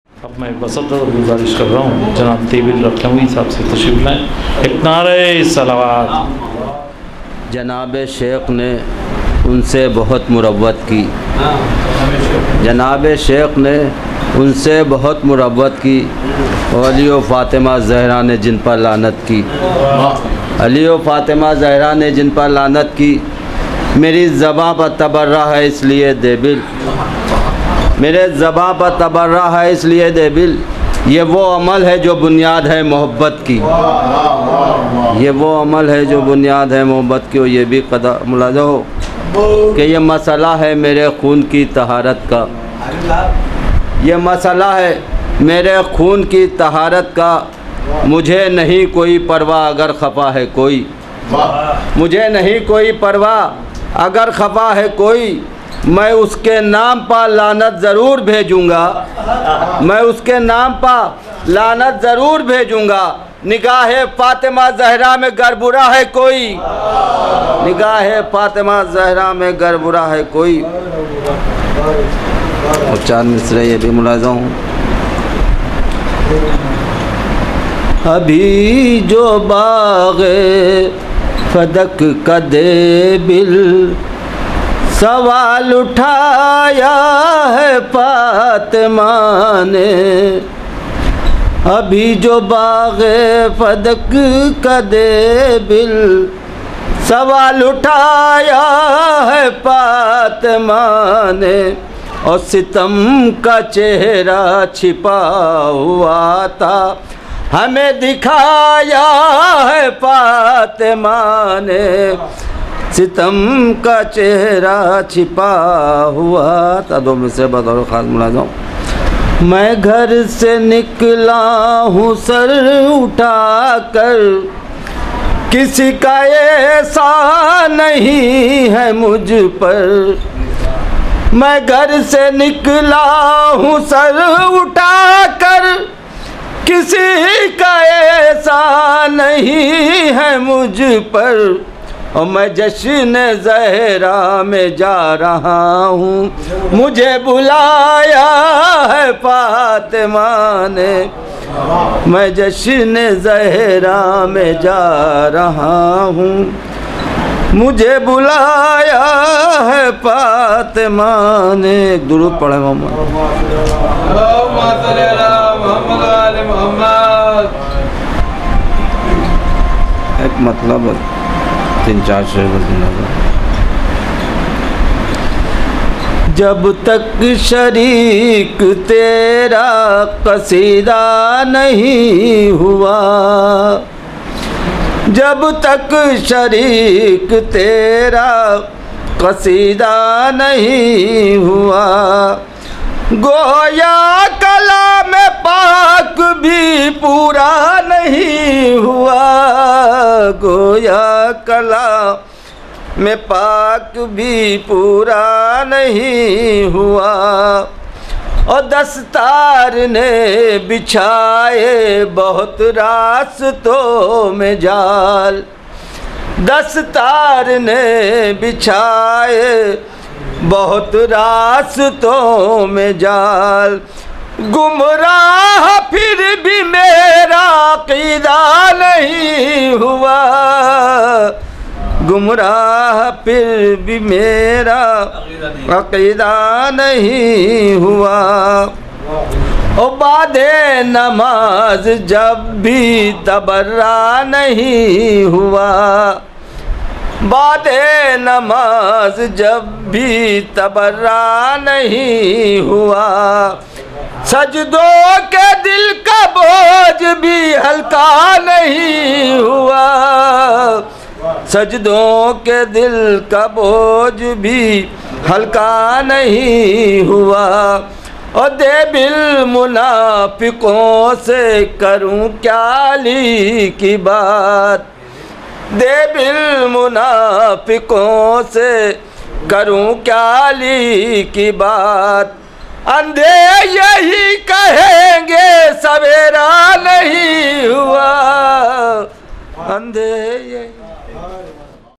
Janab Debil Lucknowi | Jashn Shahzadi-e-Kaunain 2016 | Near Masjid Mamola Lucknow
Jashn Shahzadi-e-Kaunain - 1437
On 2nd April 2016
Near Masjid Mamola Patanala Chowk Lucknow